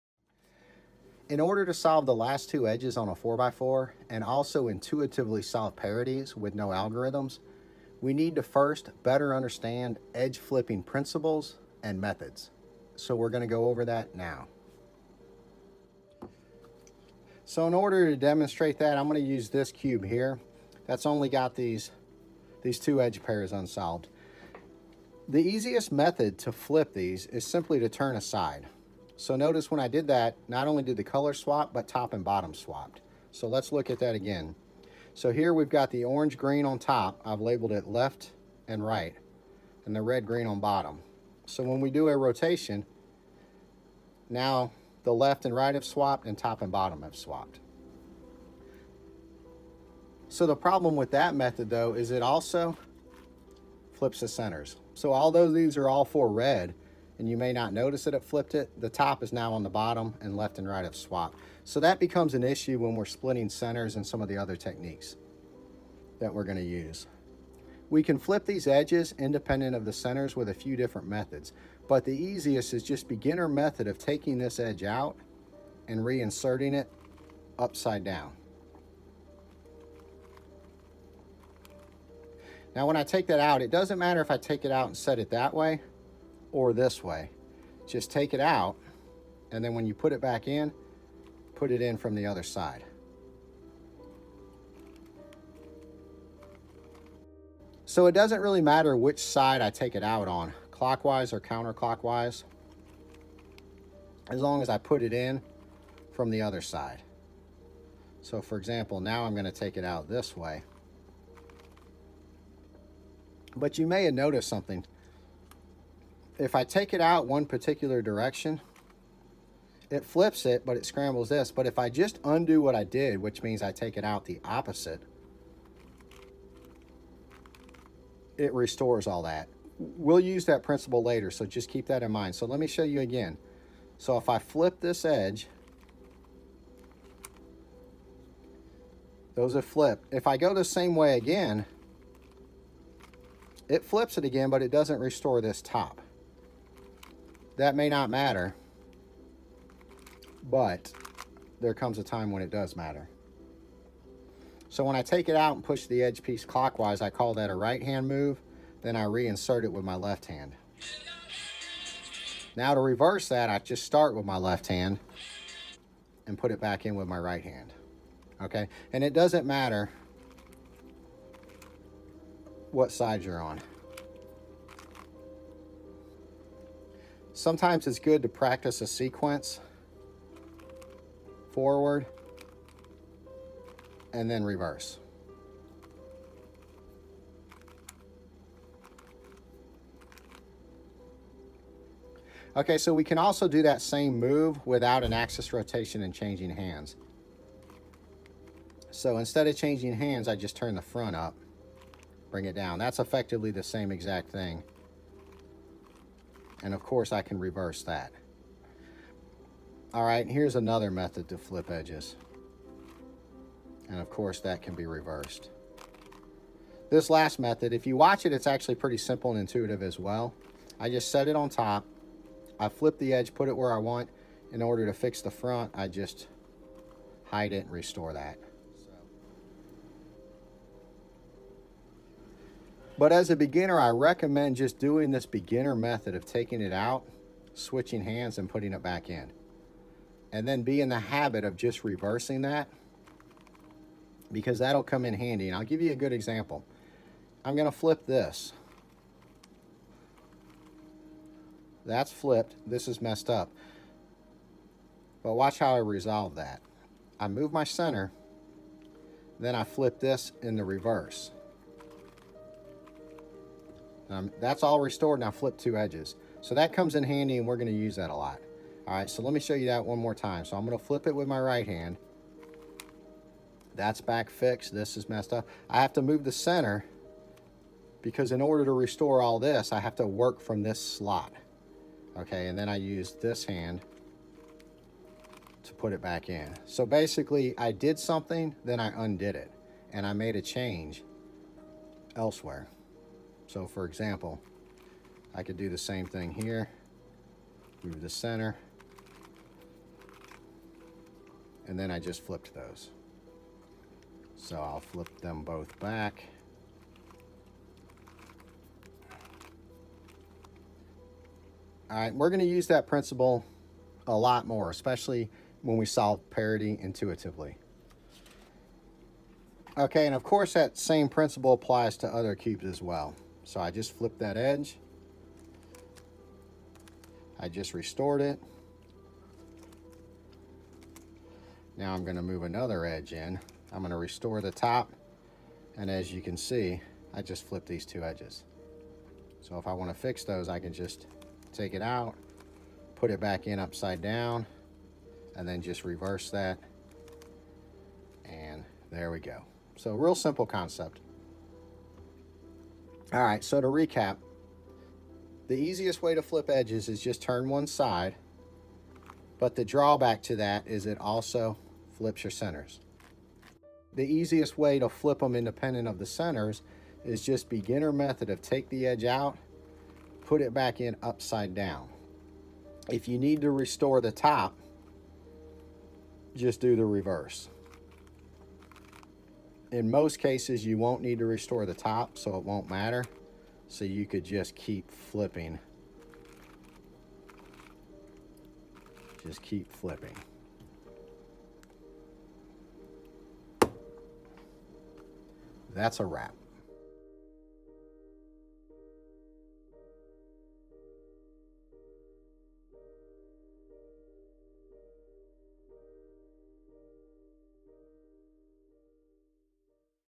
How to Solve Rubik’s Cube 4x4 Edge Flipping Fundamentals (SUP) Tutorial [KTFG 471]
How to Solve 4x4 Rubik’s Cube Edge Flipping Fundamentals: This video is part of a series of Supplemental…
This video is part of a series of Supplemental videos that provide a more detailed explanation of the Steps in my Rubik’s Cube 4x4 Tutorial, including the Theory and Principles behind the Steps, along with additional Examples and Troubleshooting tips.

*Rubik’s Cube 4x4 Edge Flipping Fundamentals (Supplemental) Tutorial [KTFG 471]*
In order to *”understand”* how to solve Rubik’s Cube Puzzle problems intuitively without algorithms, including solving 4x4 Parity, we need to better understand Edge Flipping principles.   This video instruction will help you grasp how to solve the Rubik’s Cube 4x4 intuitively without the need to memorize algorithms.  You will discover the basic principles and concepts of the Rubik’s Cube and how to recognize and solve the steps on your own instead of blindly following instructions without understanding them.

*Timestamps:*
00:00  Introduction
00:20  180 Flip
01:07  Beginner Method Flip
01:52  Restoration Trick
02:56  Practice Forward & Reverse
03:11  BM Flip Variation Front Turns
03:28  Alternate Flip Method a
03:50  Flip Methods with Notation
04:04  (Do) make a change (Undo)
05:19  Demo on 3x3
05:50  Recap
06:35  Related Videos (Subscribe)

The goal of these Tutorials is to *“Learn & Understand”*, not just copy.

Even though this video series focuses on solving the Rubik’s Cube 4x4 Intuitively without the use of algorithms, I provide a full list of relevant 4x4 Algorithms on my website & below:

*Notation for Methods of Flipping & Un-Flipping Edges*

*Beginner Method take out & re-insert upside down:*
R U R’ U’ y L’ U L  (Forward) Do
L’ U’ L U y’ R U’ R  (Reverse) Undo

*Beginner Method Variation with Front turns:*
R U R’ U’ F’ U F  (Forward) Do
F’ U’ F U R U’ R’  (Reverse) Undo

Note: R U R’ U’ y L’ U L  =  R U R’ U’ F’ U F minus axis rotation Y

*Intuitive Alternate Method a:*
R U R’ F R’ F’ R  (Forward) Do
R’ F R F’ R U’ R’  (Reverse) Undo

*Alternate Method b:*
R U’ B’ R2 (Forward) Do
R2 B U R’  (Reverse) Undo

*Alternate Method c:*
R F’ U R’ F   (Forward) Do
F’ R U’ F R’  (Reverse) Undo

Note the old notation Rr is equal to Rw (newer & more widely used)
w = wide turn (both outer & inner)
More detail on my site.

*PLL Parity Intuitive*
[u’ (Flip) u]  [d (Flip reverse) d’]
Flip = any flip method including the above methods.
Remember u & d turns are opposite each other
So u’ & d both slice to the Right (u & d’ to Left)

*PLL Parity Algorithm*
(My Method)
Rw2 R2 U2 Rw2 R2 Uw2 Rw2 R2 Uw2 U2
Alternate Method (More Efficient)
r2 U2 r2 Uw2 r2 u2

*OLL Parity Algorithm*
Rw2 B2 U2 Lw U2 Rw’ U2 Rw U2 F2 Rw F2 Lw’ B2 Rw2